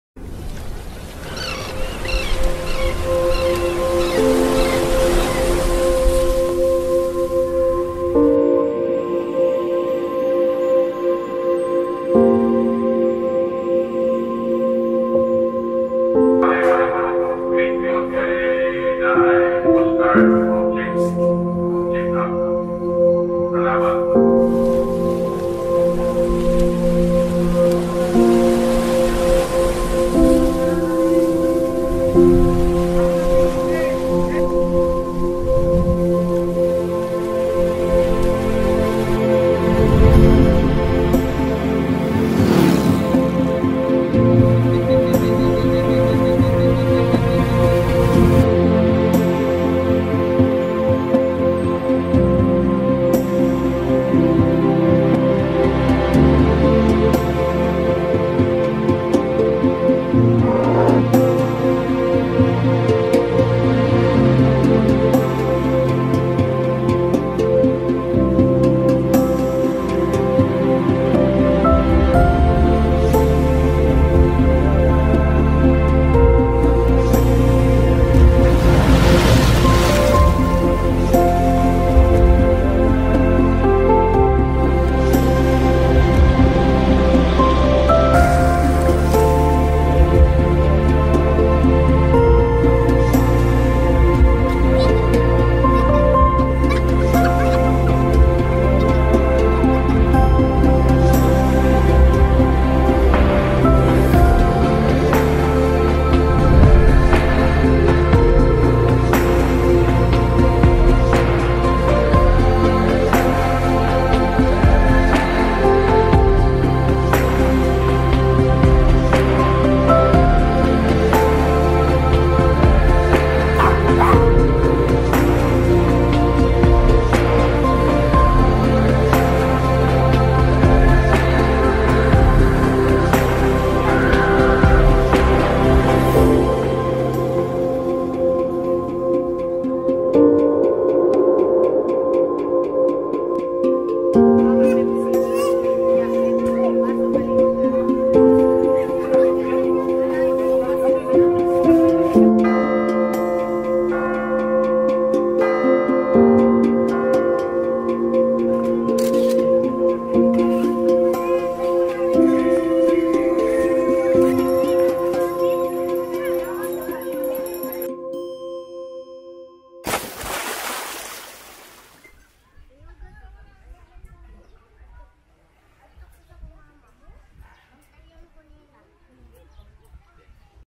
A Travel Short Film ft. the beautiful Bantayan Island, Cebu, Philippines
Bantayan Island is a popular island destination in the province of Cebu in the Philippines. It boasts of white sand beaches, lagoons, a mangrove forest and dozens of resorts to choose from. Some notable spots in the island that are hard to miss are Kota Beach, Balidbid Lagoon, Oboob Mangrove, Sandira Beach, Sugar Beach, Diving Cliff, Nature Park, Kota Park and Ogtong Cave.